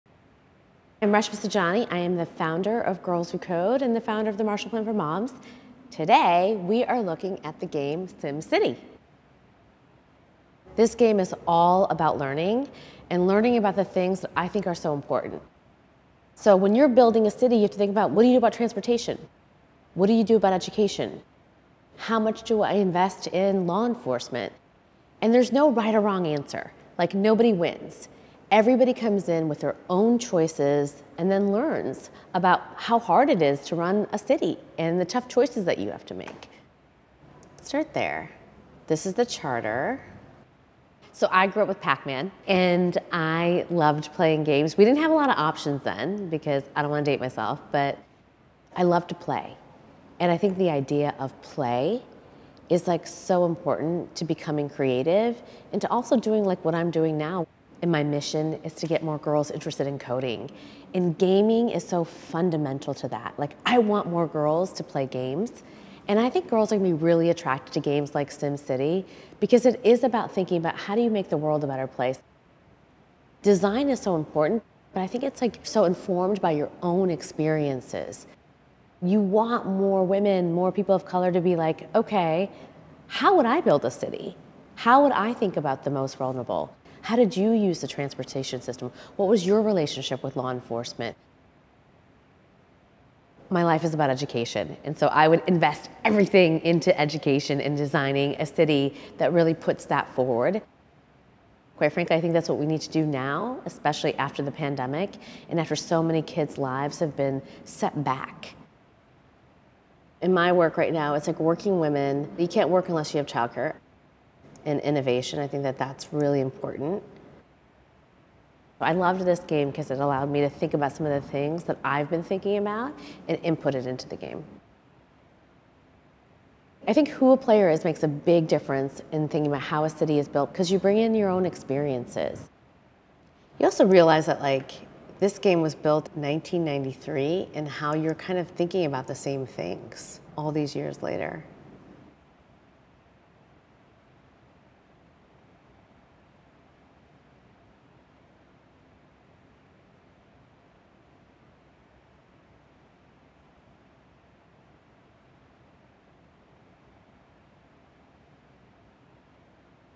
How would you build a city? Girls Who Code founder Reshma Saujani on SimCity | UNIQLO ArtSpeaks
Best-selling author and founder of Girls Who Code, Reshma Saujani, talks about the realities of building a virtual world in SimCity and how video games can be much more than entertainment.

Commit to art and ideas. Support MoMA by becoming a member today

The comments and opinions expressed in this video are those of the speaker alone, and do not represent the views of The Museum of Modern Art, its personnel, or any artist.